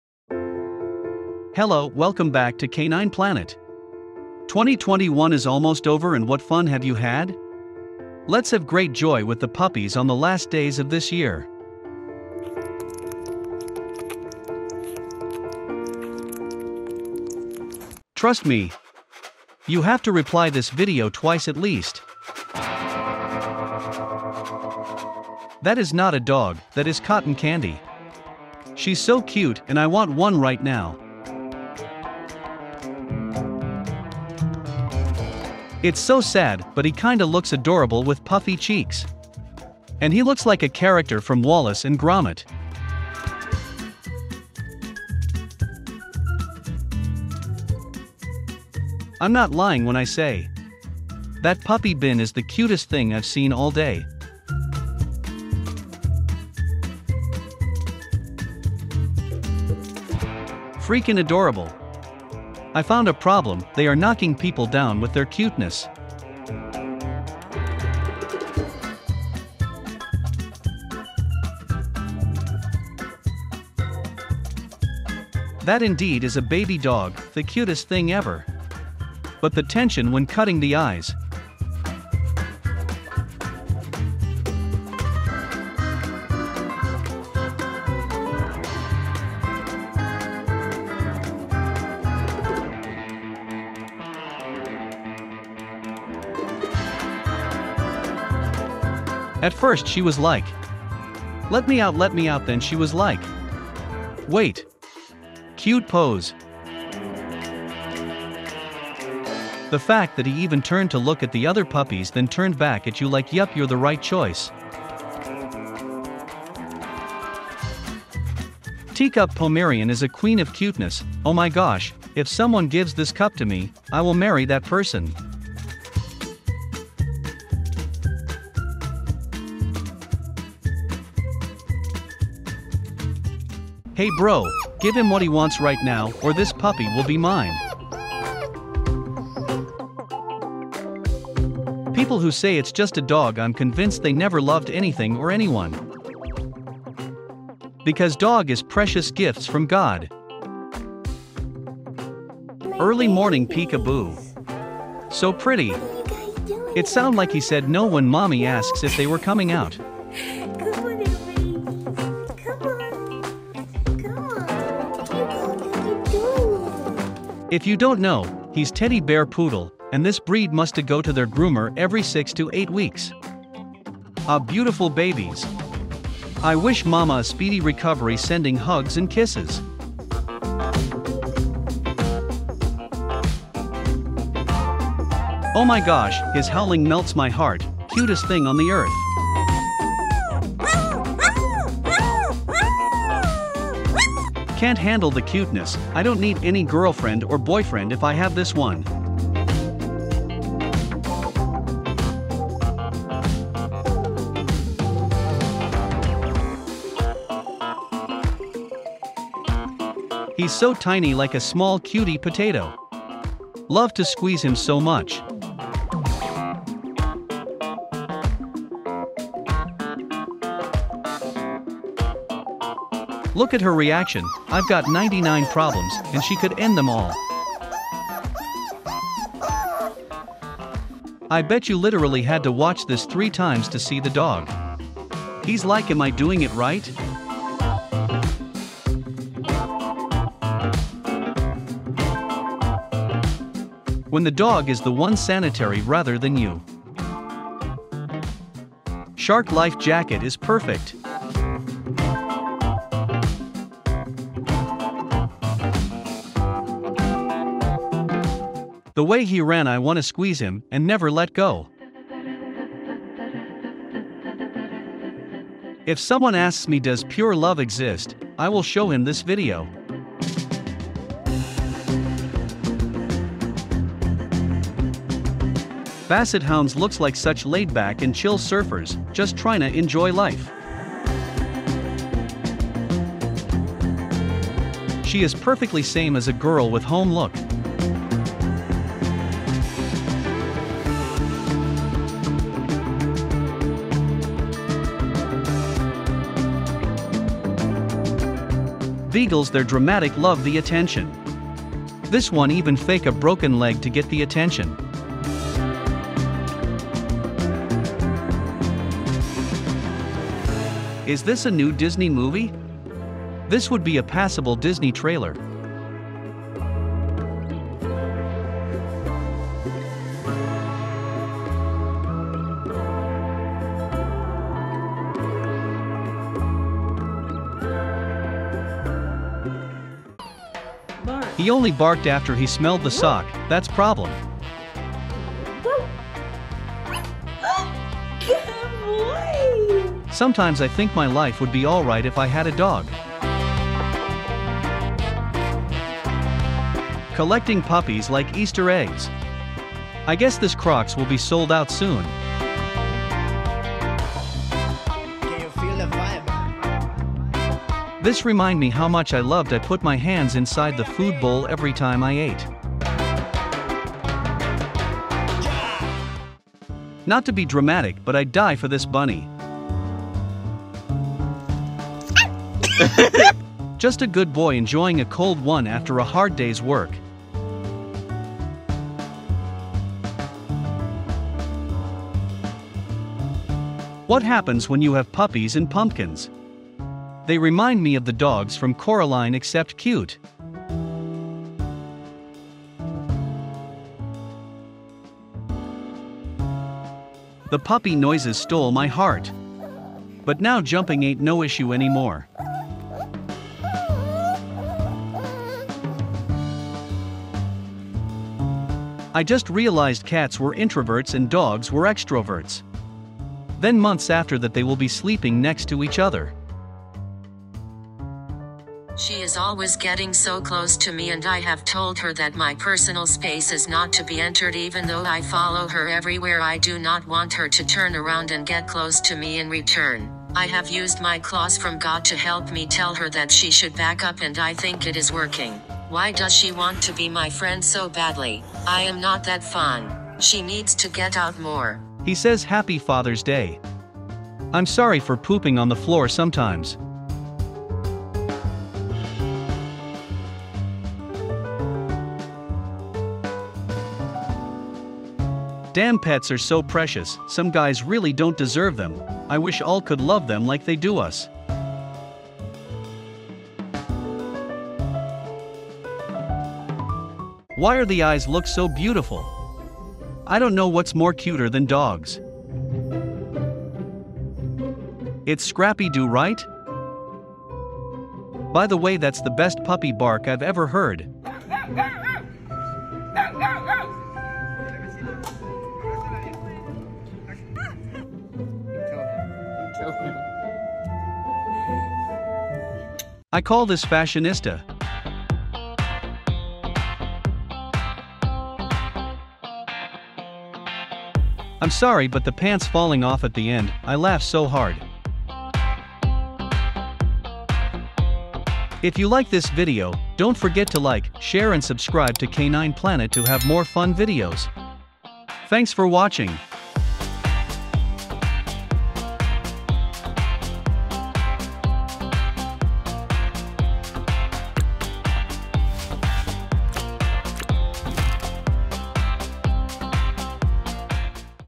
Puppies Are Cutest On Earth #15 | These Puppies Will Make Your 2021 Rosy
Hello, welcome back to Canine Planet. 2021 is almost over and what fun have you had? Let's have great joy with the puppies on the last days of this year.
🌞 There is a fact you may not know that puppies can make people fall in love with their cuteness. Wanna see the cutest puppy attacks? Let's go with Canine Planet!
Most of our content has been authorized by the owners. For some videos, We have not been able to contact owners yet.